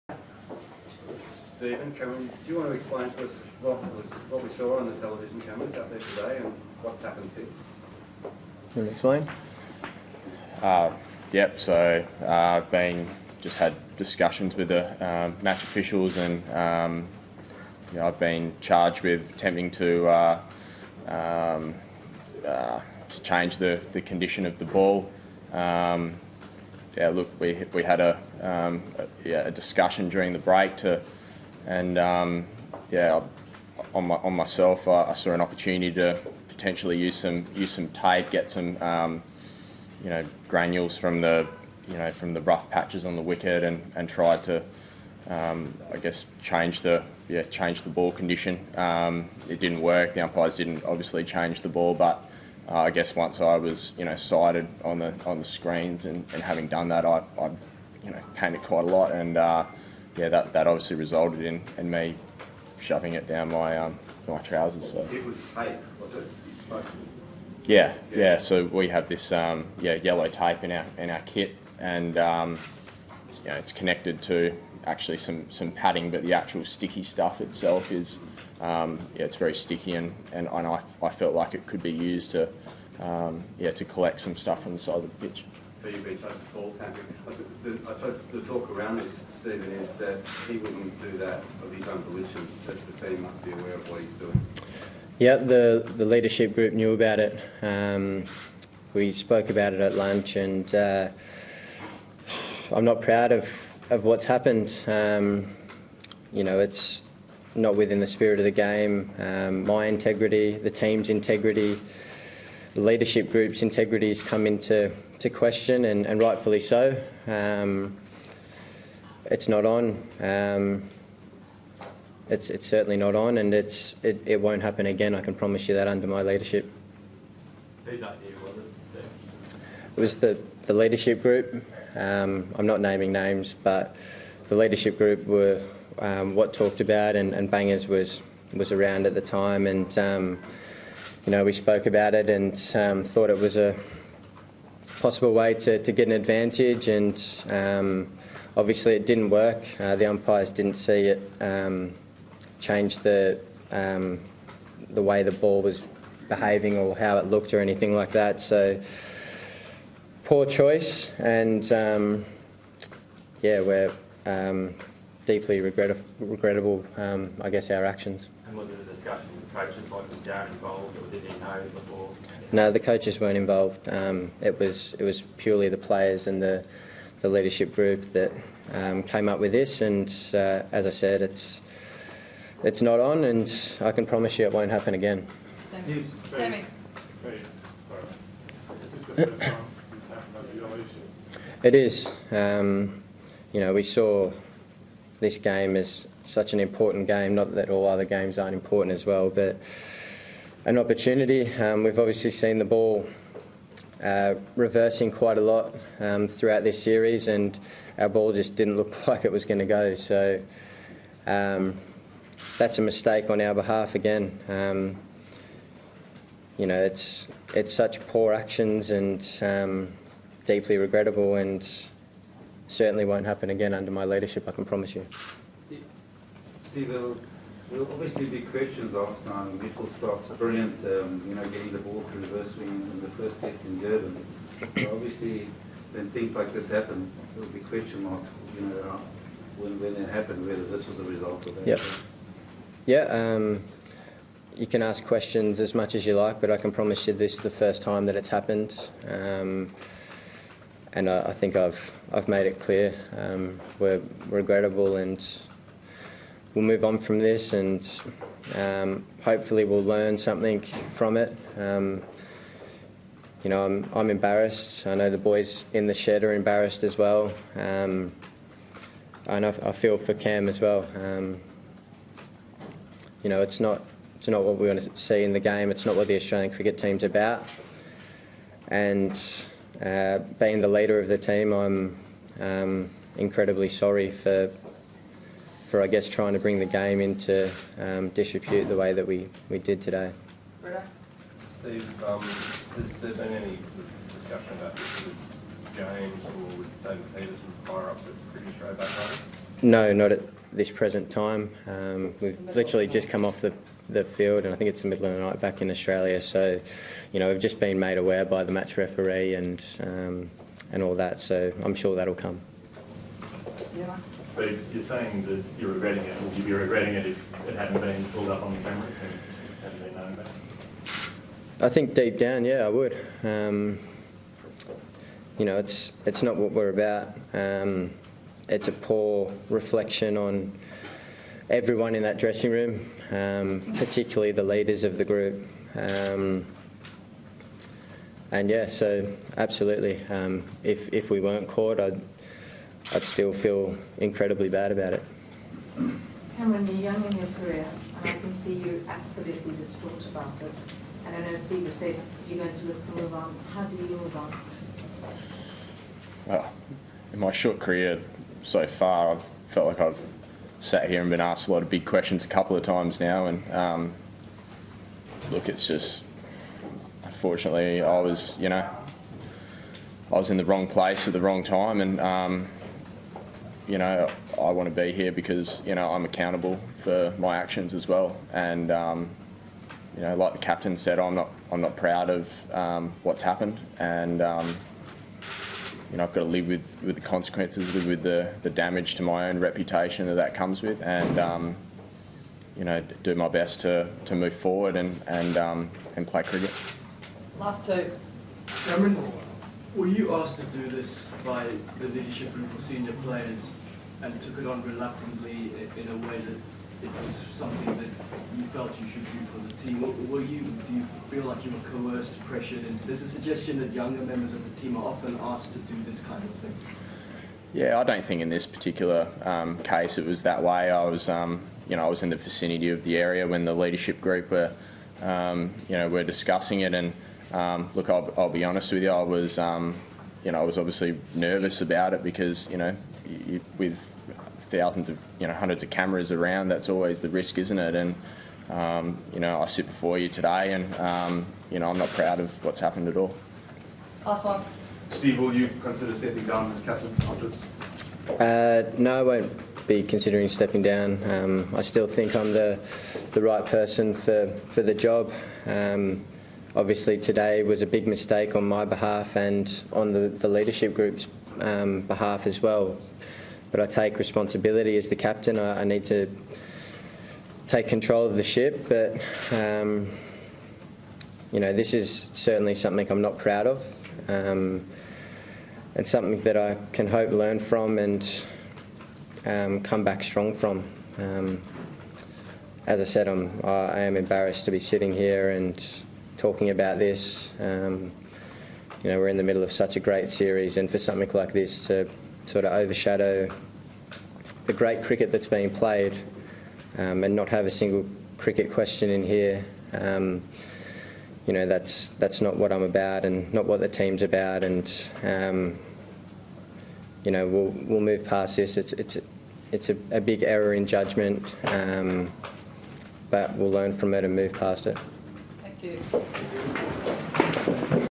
Steve Smith & Cameron Bancroft interview after ball tampering incident || HD
Press conference of Australian caption after ball tampering incident .